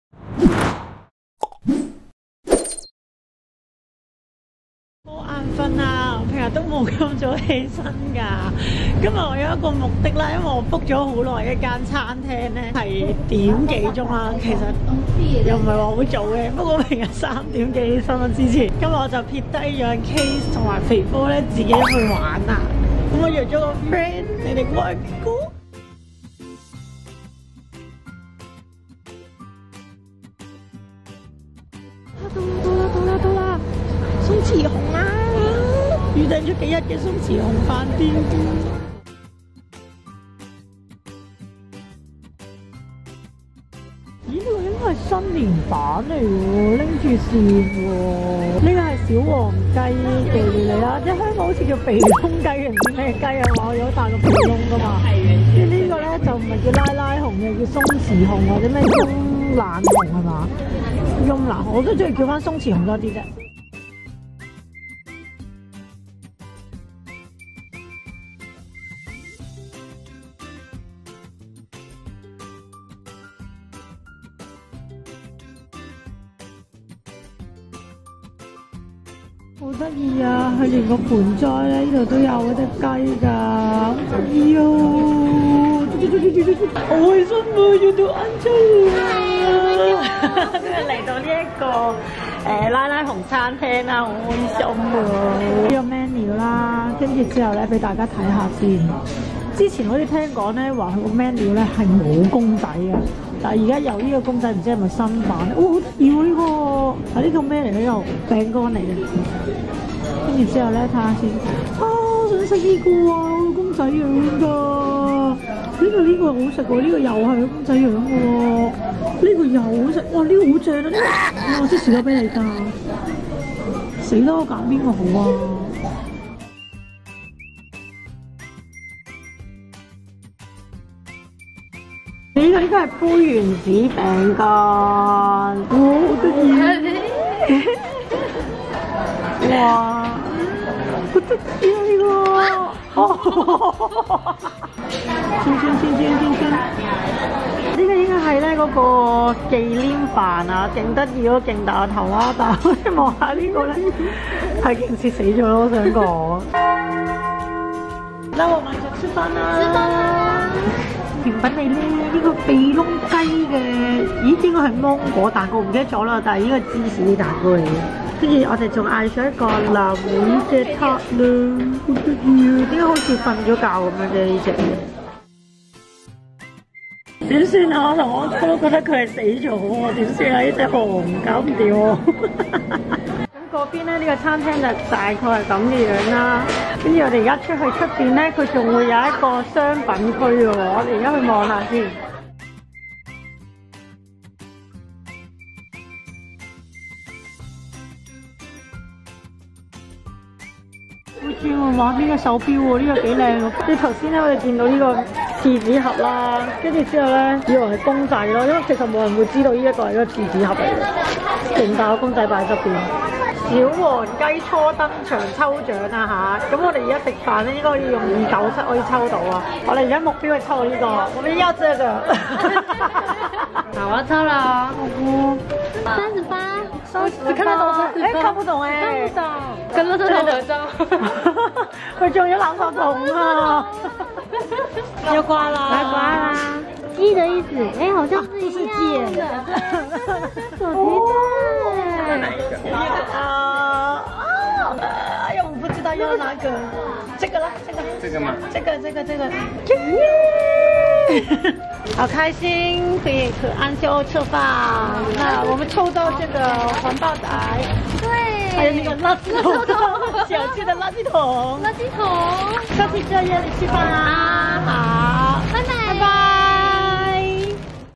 和安啾一起玩囉~ [台北拉拉熊餐廳] (Vlog) 台北2017#6
KZee同肥波都去左電玩展~
一於乘機找安啾玩！
Yeah!

ლ(◉◞౪◟◉ )ლ